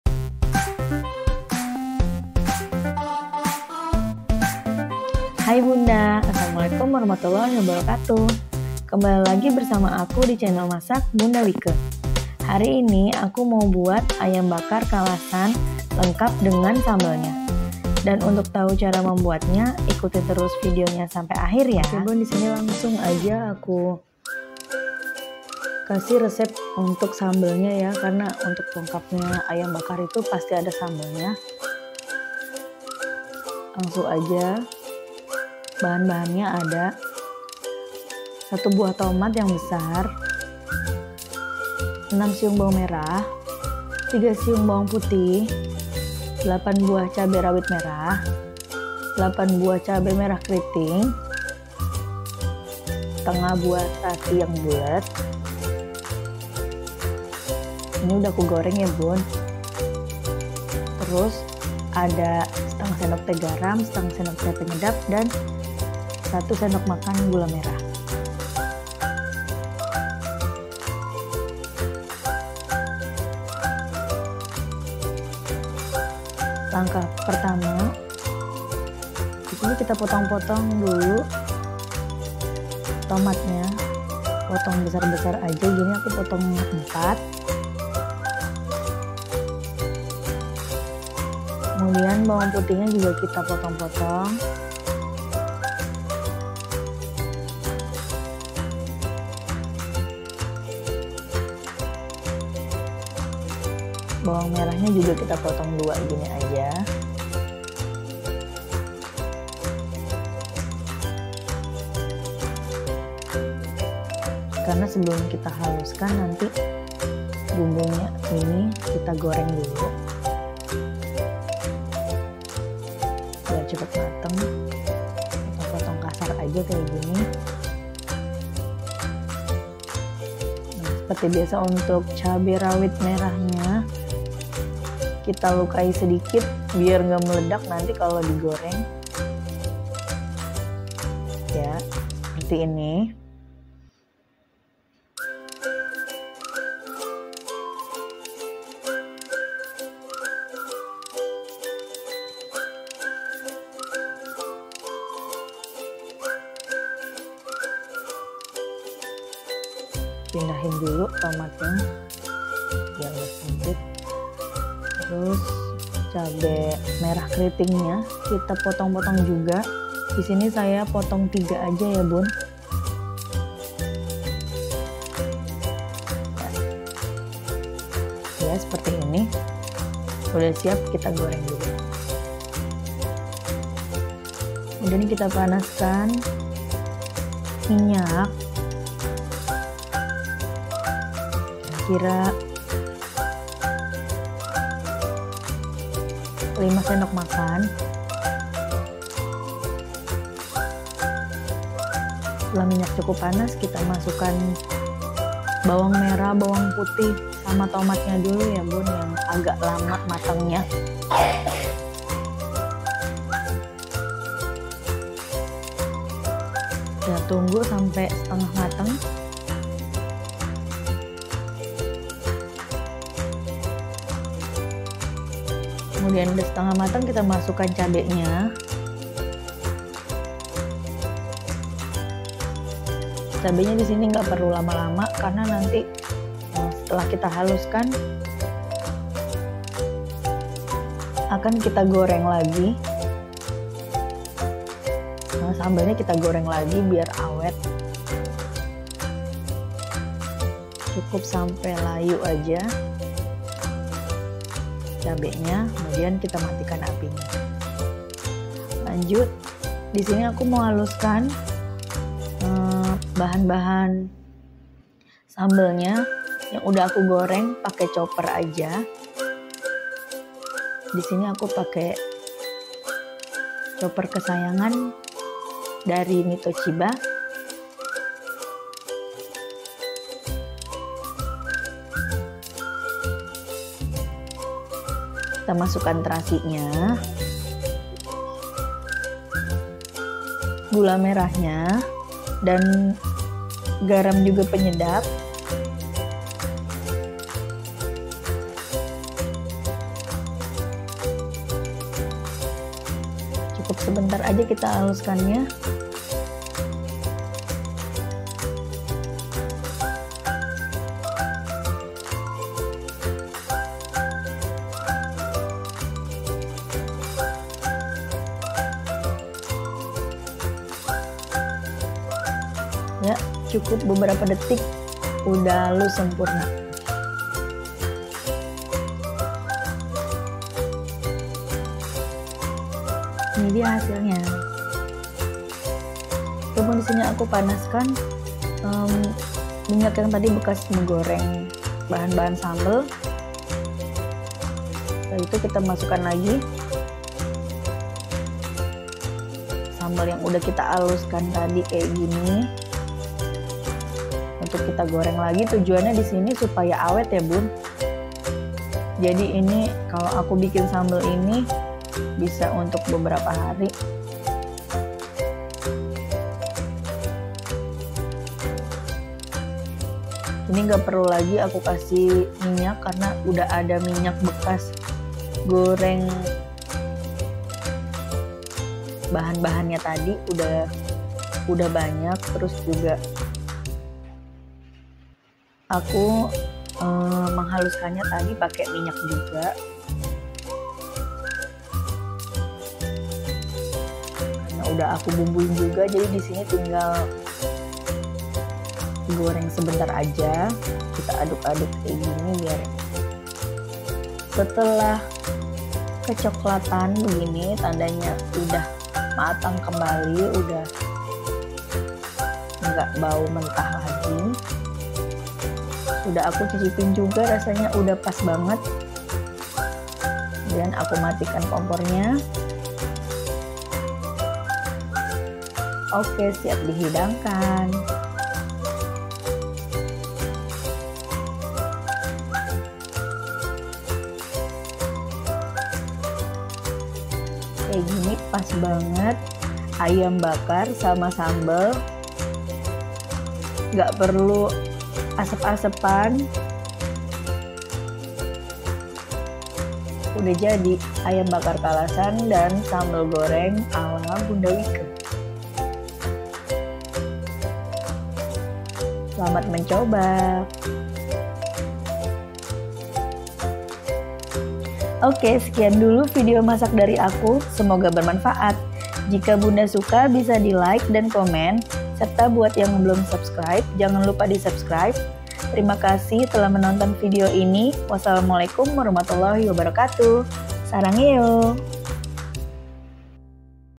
Resep Sambel goreng terasi pelengkap ayam bakar kalasan Bunda Whike
Hai Bunda apa kabar?
kali ini aku mau buat sambel goreng terasi untuk pelengkap ayam goreng kalasan. Selain bisa dijadikan ide menu masakan sehari hari untuk keluarga tercinta, ayam bakar kalasan dan sambel goreng terasi ini bisa juga untuk ide jualan ya bun. resep yang simpel, praktis, mudah dibuat dan yang pasti enak. Yuk belajar masak untuk orang orang tercinta.
langsung saja bahan bahan yang aku pakai adalah sebagai berikut :

Bahan - Bahan :
1 buah tomat ukuran besar
6 siung bawang merah
3 siung bawang putih
8 buah cabe rawit merah
8 buah cabe merah keriting
1/2 buah terasi bulat
1 sdt garam
1/2 sdt penyedap
1 sdm gula merah
secukupnya minyak goreng